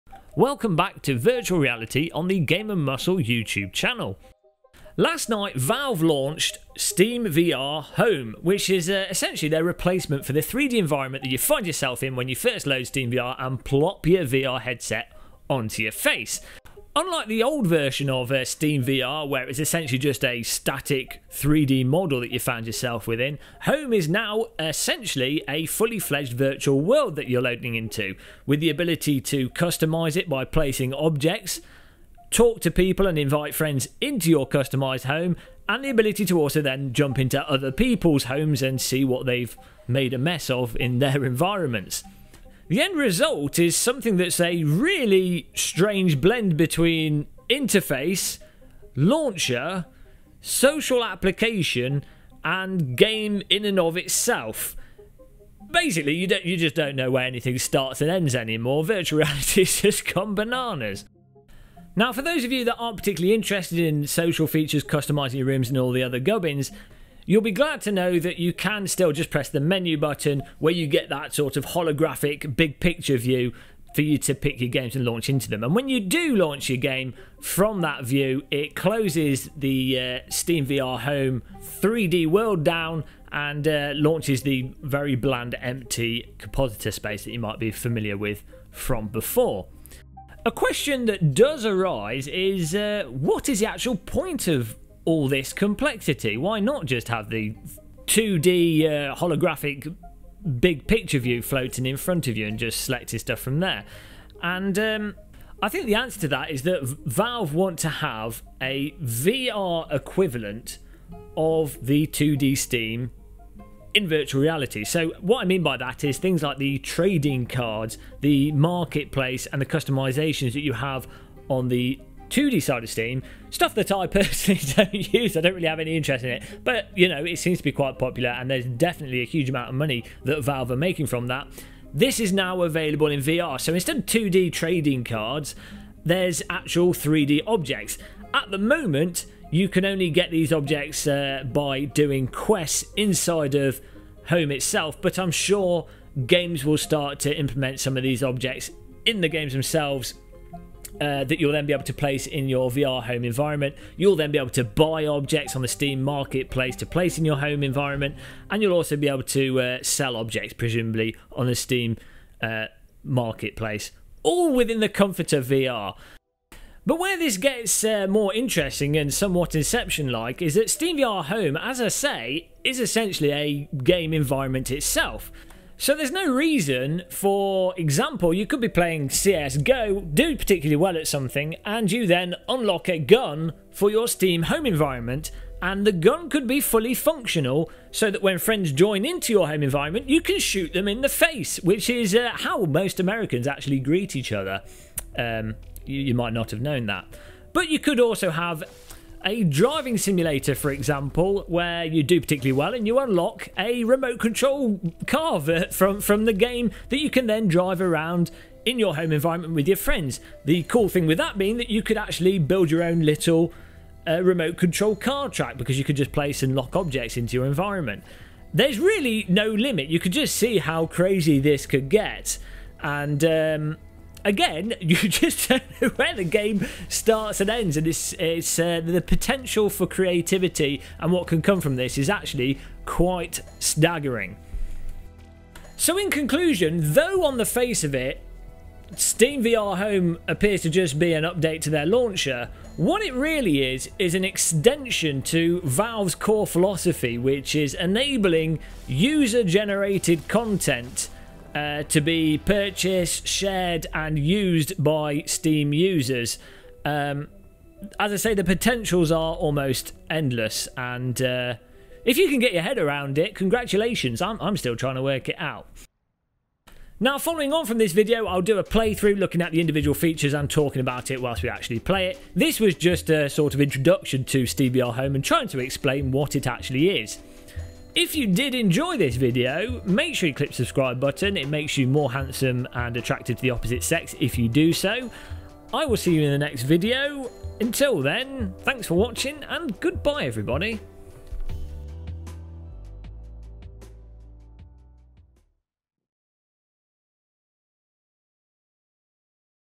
WHAT IS STEAM VR HOME ?
What is steam VR Home ?  In this video we take a look at steam VR home which was just launched with the latest steam VR Beta client and try to understand what valve are doing with there new Home platform.

If you want to try out steam VR home for yourself just make sure you select the BETA version of Steam VR from the tools tab inside of steam and then launch steam VR.

Steam VR Home will work with any open VR compatible headset and can be used game pads, steam or oculus touch controllers.

-- From the developer --

Today we are updating SteamVR with a new home experience that is richer, more interactive, and more social than the existing launch area. SteamVR Home is currently in beta and will appear for everyone who opts in to SteamVR Beta.

Ever since SteamVR was released last year, we've been steadily adding ways to customize the launch area (where players start when starting up SteamVR). It's been exciting to see the creativity of the community in the diverse environments and backgrounds submitted to the SteamVR Workshop so far. We've also heard loud and clear that creators want the ability to make more detailed home environments, with sound, animation, interaction, and social elements. All of these features and more are now available with the new SteamVR Home update.